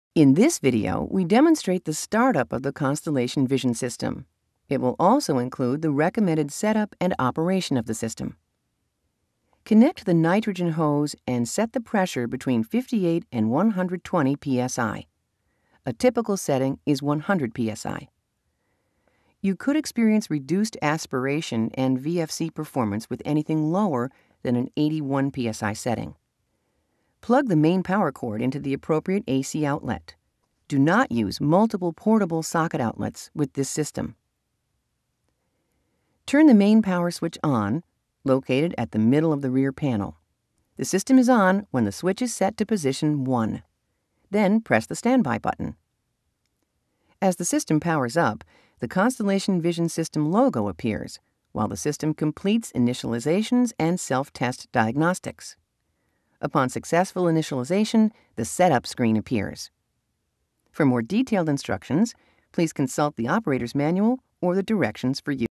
Alcon Constellation System Start Up
Overview of the Alcon Constellation Start up.  CR3 software Version .  Newer version has the Doctor and Procedure combined.